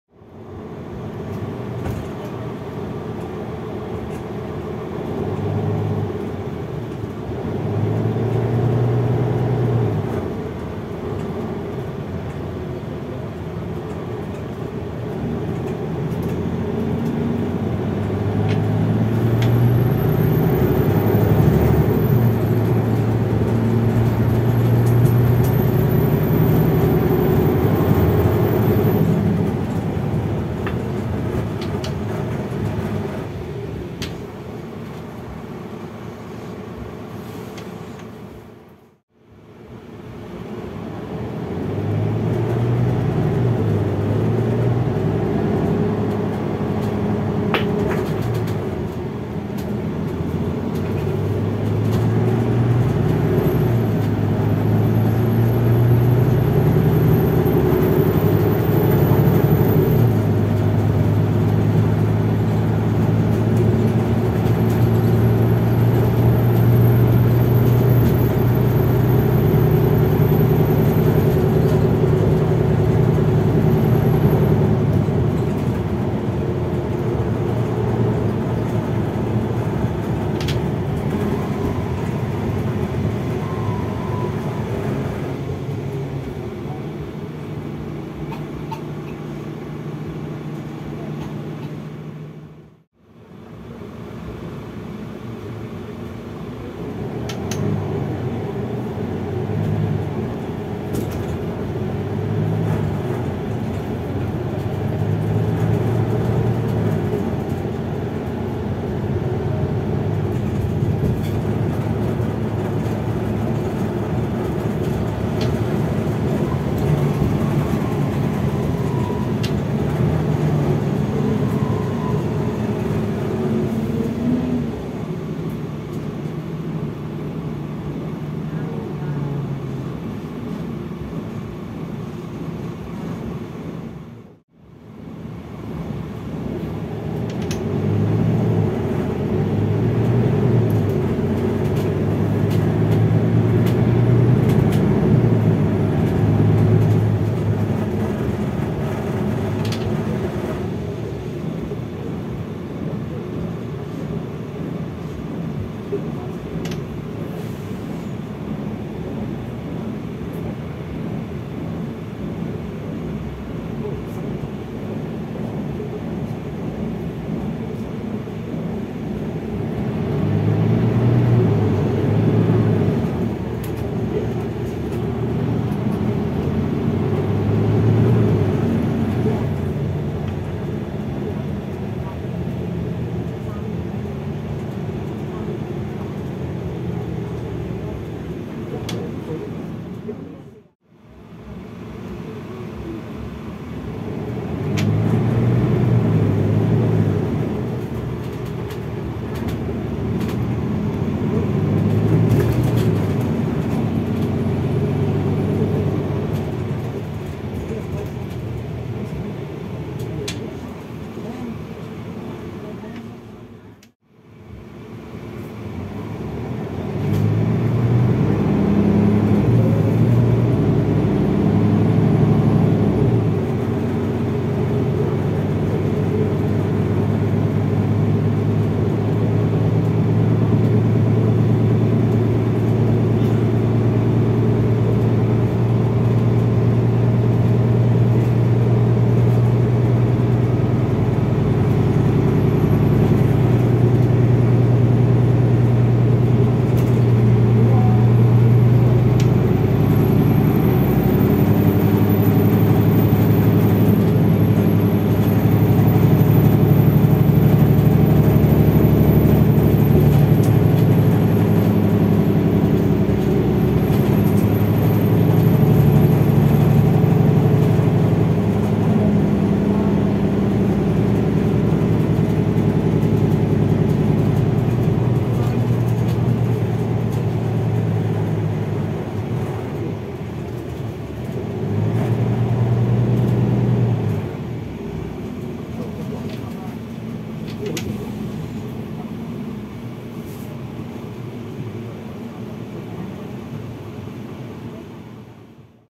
[Bus Audio] Argos Bus PH2795@HIRE (MAN RR8 | ZF Ecolife)
Bus Information:
Engine: MAN D1556LOH-16 (Euro 6d, 360hp)
Gearbox: ZF Ecolife 6AP1400
Date of Registration: 2022/12/__
Bodywork: Gemilang Lion's Intercity
VIN: TBC